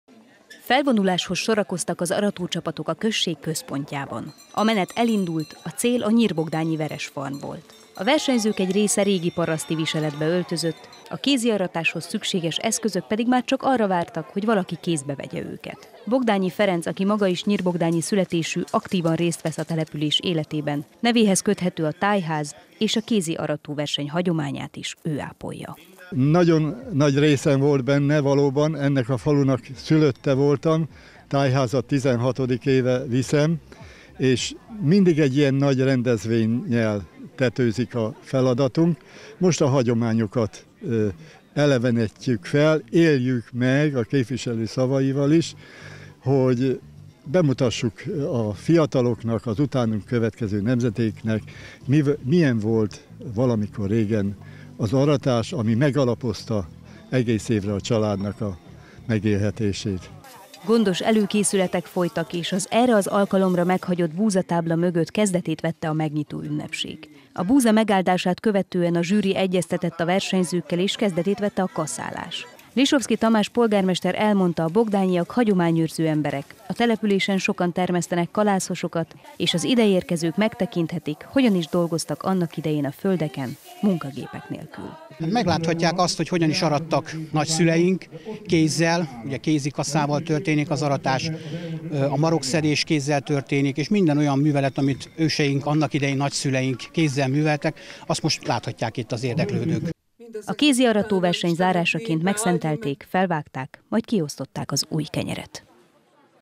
Kézi aratóversenyt szerveztek Nyírbogdányban
Tizedik alkalommal mutatták meg Nyírbogdányban, hogy hogyan kell kézzel aratni. A hétvégi versenyen az volt a cél, hogy a csapatok hagyományos módszerekkel takarítsák be a terményt. A szokásokhoz hozzátartozott, hogy aratás előtt megáldották a búzát.